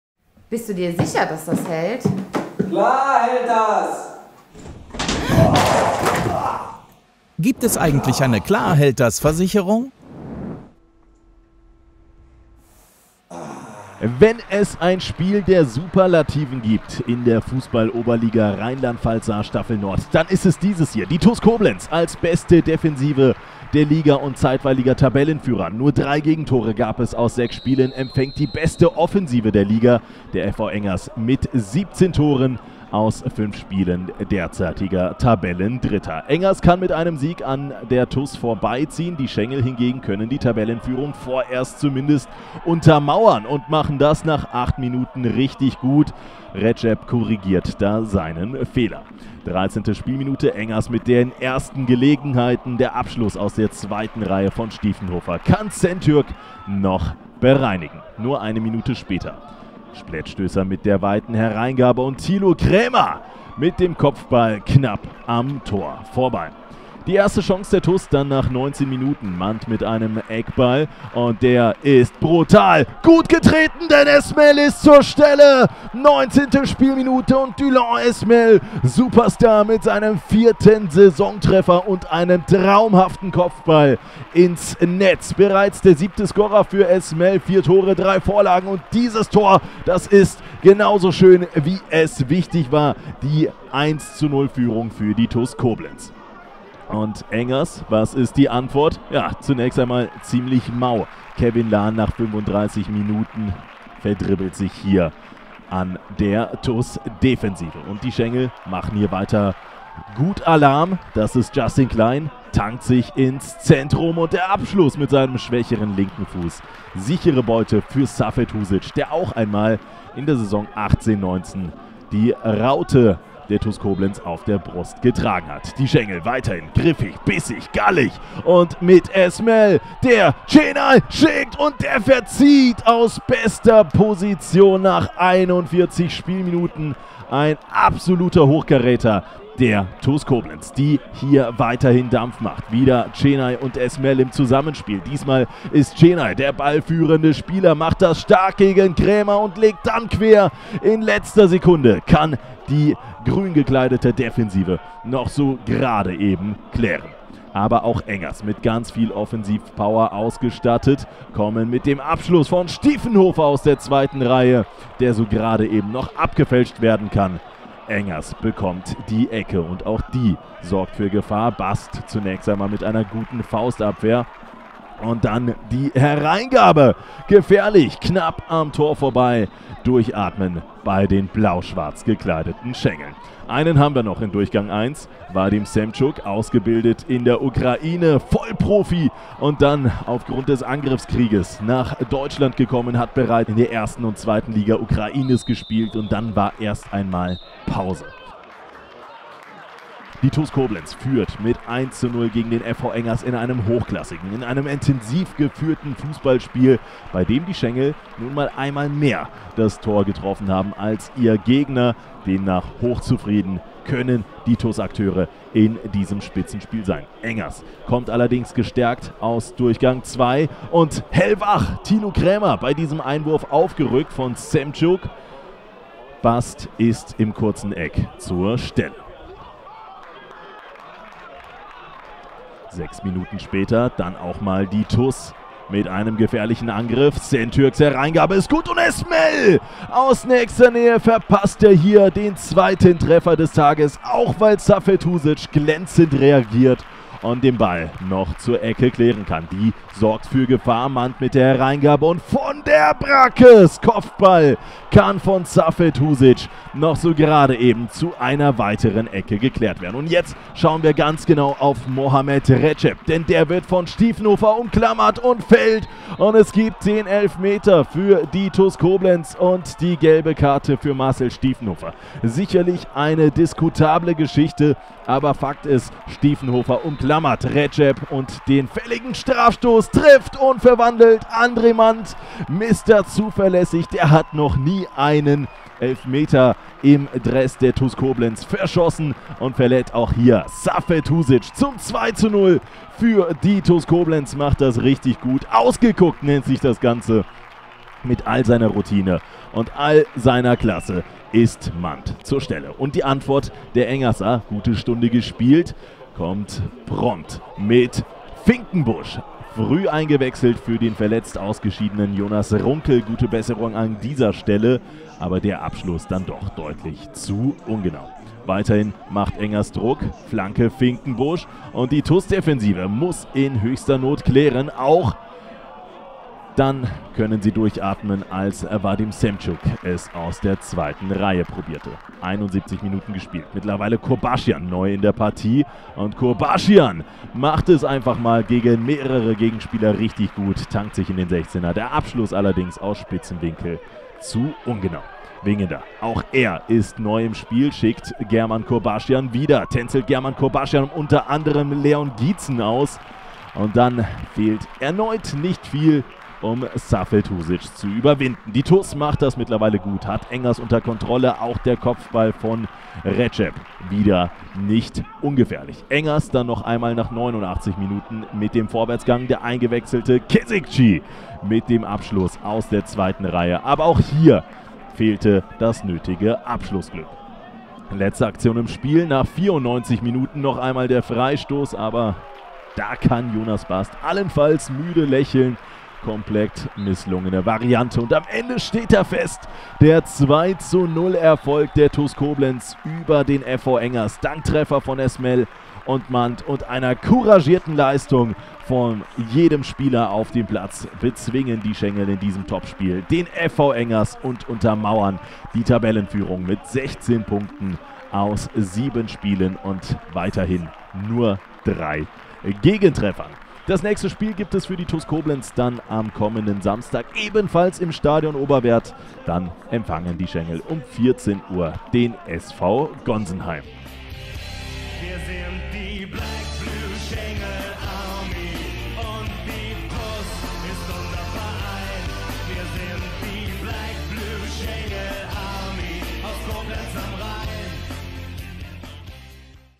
Highlights | TuS Koblenz - FV Engers 07 2:0 | 7. Spieltag | Oberliga RP/S - Nord | 2022/2023
TuS Koblenz schlägt FV Engers in einer hochklassigen Partie mit 2:0

Die Ausgangssituation versprach im Vorfeld der Partie zwischen der TuS Koblenz und dem FV Engers so einiges: Die beste Defensive der Nordstaffel (TuS Koblenz - drei Gegentreffer in sechs Partien) traf auf die bis dato beste Offensive der Staffel (FV Engers - 17 Tore in 5 Partien), der vorläufige Tabellenerste auf den Tabellendritten.

Und die Begegnung zwischen den Schängeln und den Jungs vom Wasserturm sollte halten, was sie versprach: Schneller Fußball auf beiden Seiten und einen offenen Schlagabtausch zwischen den Mannschaften, sahen die 958 Zuschauerinnen und Zuschauer im Stadion Oberwerth am vergangenen Samstag.

Den besseren Start in die Partie erwischten die Neuwieder: Während die ersten Annäherungen auf das Tor von Jonas Bast noch eher ungefährlich waren, kam Engers in Minute 14 dann das erste Mal richtig aussichtsreich vorm Koblenzer Tor zum Abschluss. Der Kopfball vom Kapitän des FV Engers, Thilo Krämer, verpasste den Koblenzer Kasten nur knapp.

Die erste Chance der TuS Koblenz dann nach 19 Minuten: Der von André Mandt hereingetretene Eckball segelte in Richtung Fünfmeterraum und fand den Kopf von TuS-Offensivmann Dylan Esmel, der den Ball wuchtig im Tor der Neuwieder unterbrachte, keine Chance für Schlussmann Safet Husic und das 1:0 für die TuS Koblenz.

Insbesondere in der Phase vor der Halbzeit kam es dann zum offenen Schlagabtausch zwischen beiden Mannschaften: Der Abschluss von Justin Klein in der 39. Minute war zwar kein Problem für Torhüter Safet Husic, spätestens in Minute 41 wäre der Engeres Keeper aber vermutlich machtlos gewesen, als Dylan Esmel Armend Qenaj mustergültig im Sechzehner bediente, doch der Abschluss von Armend Qenaj rauschte aus aussichtsreicher Position am Tor vorbei. Auch in Minute 43 sorgte das Zusammenspiel von Qenaj und Esmel für Gefahr, konnte aber von der Defensive des FV Engers geklärt werden.

In den letzten zwei Minuten vor dem Pausenpfiff kamen die Neuwieder dann gefährlich vor das Koblenzer Tor, doch sowohl der Abschluss von Stieffenhofer (44. Minute), als auch die Torannäherungen von Jonathan Kap und Vadym Semchuk führten nicht zum Torerfolg für die Grün-Weißen, so dass es mit einem 1:0 für die Schängel in einer eher ausgeglichenen ersten Halbzeit in die Pause ging.

Direkt nach dem Anpfiff kamen die Engerser erneut zu einer guten Chance, doch fortan übernahmen die Koblenzer immer mehr das Kommando: Eine Sentürk-Heingabe konnte Esmel, vor Husic alleinstehend aufgetaucht, in Minute 52 nicht verwerten, auch ein gefährlicher Kopfball von Daniel von der Bracke konnte von Husic glänzend geklärt werden (53. Minute).

In Minute 54 dann das, was der Trainer des FV Engers 07, Sascha Watzlawik, in der anschließenden Pressekonferenz als „Genickbruch“ für seine Truppe bezeichnete: Nach einer Mandt-Ecke, wurde TuS-Verteidiger Redjeb geklammert und ging zu Boden und der Schiri, gut postiert, zeigte direkt auf den Punkt. Für Watzlawik eine Fehlentscheidung, die Entscheidung des Schiedsrichters stand jedoch. Und André Mandt, zuverlässig vom Punkt wie immer, verlud Husic und erzielte in Minute 55 damit den 2. Treffer für die Schängel an diesem Samstagnachmittag.

Die Engerser warfen jetzt alles nach vorne, ohne aber den Defensivverbund der Schängel, der schon in den bisherigen Saisonspielen kaum zu überwinden war, zu knacken. Das System der Schängel, jetzt mit 2:0 in Führung, wurde defensiver jetzt noch kompakter und man verlegte sich aufs Kontern. Die sich ergebenden Räume blieben aber ungenutzt: Der eingewechselte German Kurbashyan scheiterte zweimal (Minute 71 und 78), auch der Kopfball von Redjeb (85. Minute) fand nicht den Weg ins Tor.

Und so hieß es an diesem Nachmittag: Kein Durchkommen für die bis dato beste Offensive der Nordstaffel, die beste Defensive der Staffel samt hervorragend aufgelegtem Torhüter Jonas Bast hielten den Kasten sauber, im Gegenzug überzeugten die Offensivaktionen der Schängel.

Und während sich das Spiel in Halbzeit eins eher ausgeglichen gestaltete, übernahm die TuS in Halbzeit zwei die Spielkontrolle und gewann die hochklassige und spannende Begegnung gegen einen starken Gegner verdient mit 2:0.

Das nächste Spiel der Schängel ist am kommenden Samstag, dem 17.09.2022. Dann trifft man um 14:00 Uhr im Stadion Oberwerth auf den SV Gonsenheim. Die TuS Koblenz freut sich über eure Unterstützung!

Für die TuS Koblenz spielten:
Bast - von der Bracke, Stahl, Redjeb - Klein, Grgic (59. Wingender), Mandt, Sentürk - Hadzic (83. Mahrla), Qenaj (68. Kurbashyan) - Esmel (68. Pistor)

Tore:
1:0 Dylan Esmel (19.)
2:0 André Mandt (55. / FE)

Besondere Vorkommnisse:
Rote Karte: Sascha Watzlawik (Trainer FV Engers 07 / 55.)

Zuschauer:
958

Unterstütze das TuS TV und die Digitalisierung der TuS Koblenz